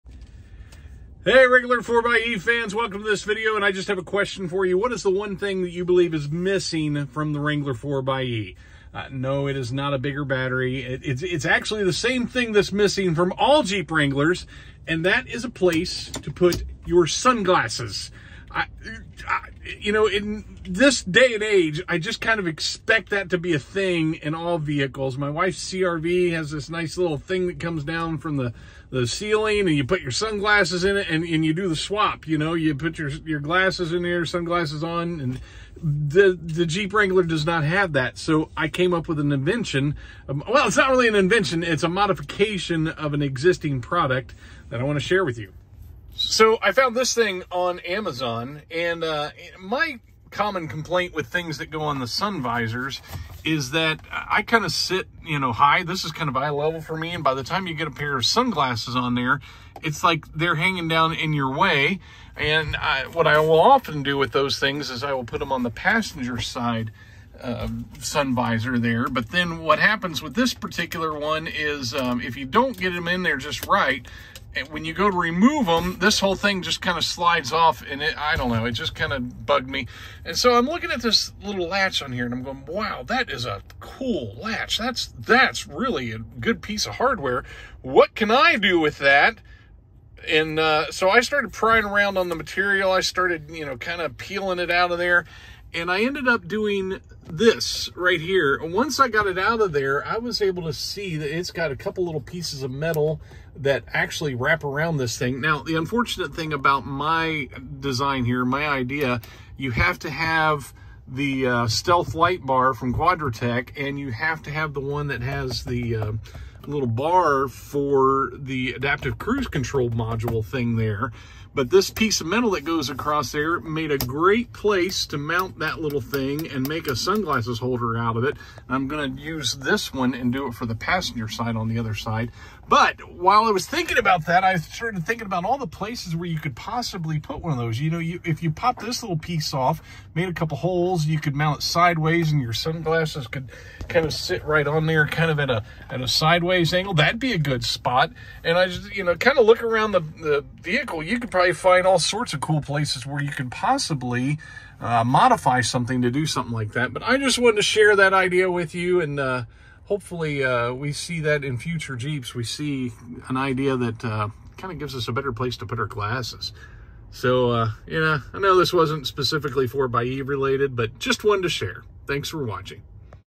Sunglasses storage solution
I made my own storage solution for my sunglasses and wanted to share.

Here is the link to the product I modified